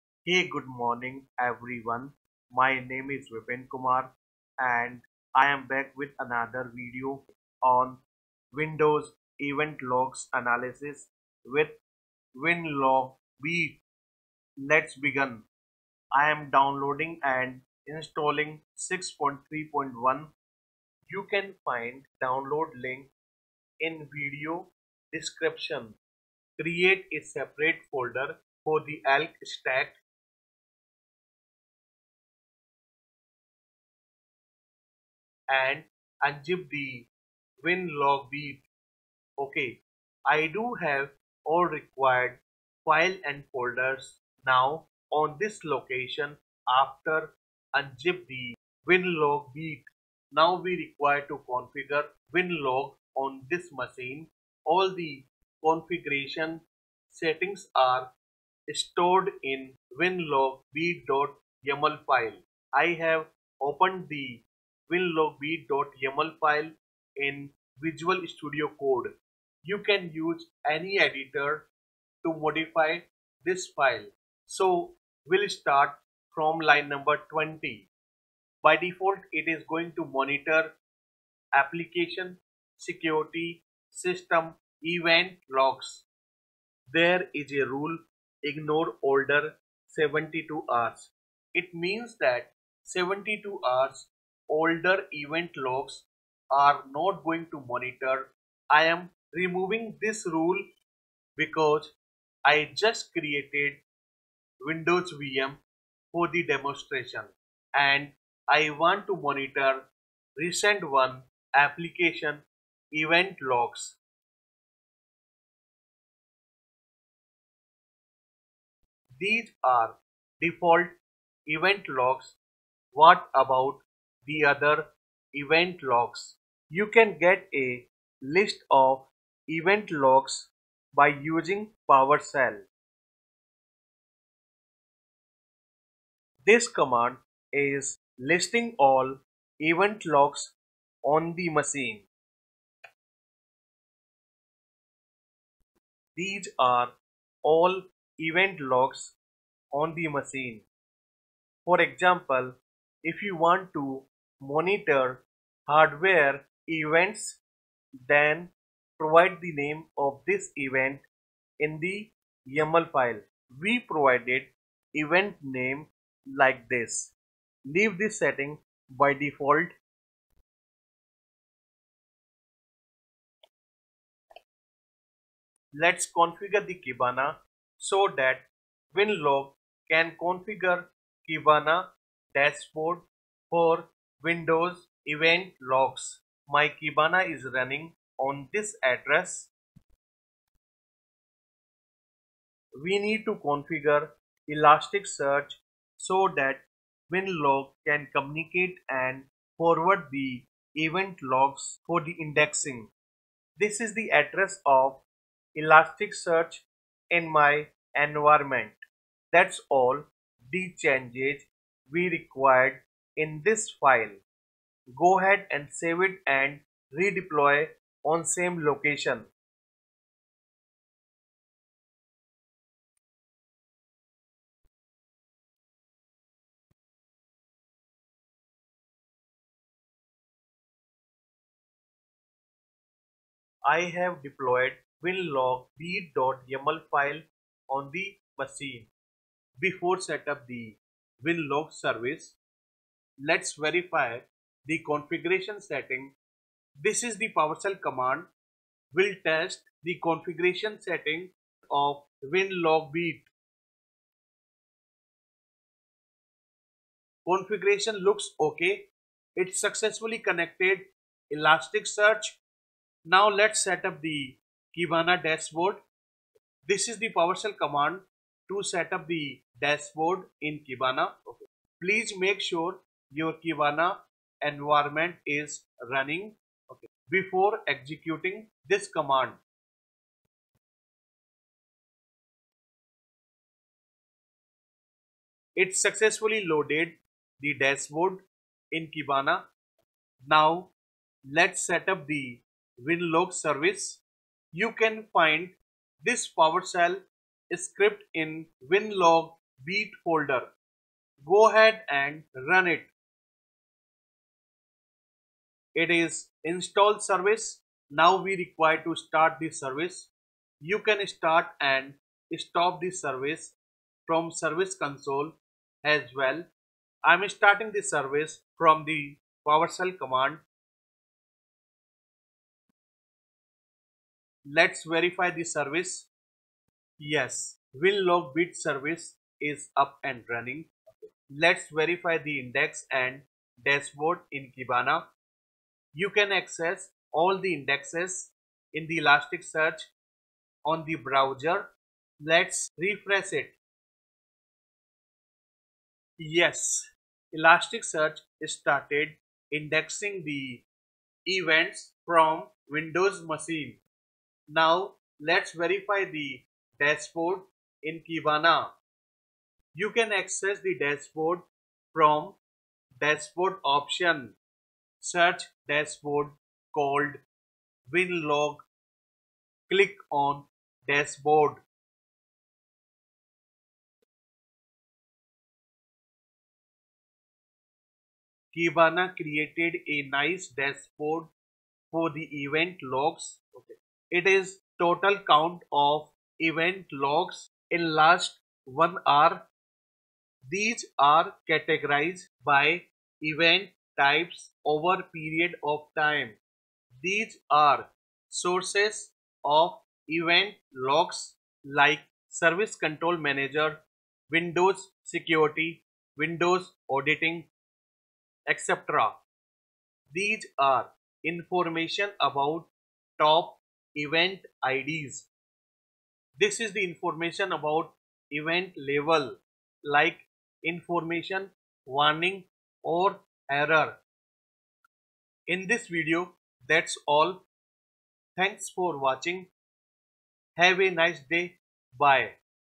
ELK Stack - Windows Event Logs Analysis using Winlogbeat
"Winlogbeat" installation and configuration.
Configure "winlogbeat.yml" file.
Windows Event Logs forwarding to elasticsearch.
Kibana Event Dashboard.
Windows Event Logs discover in Kibana.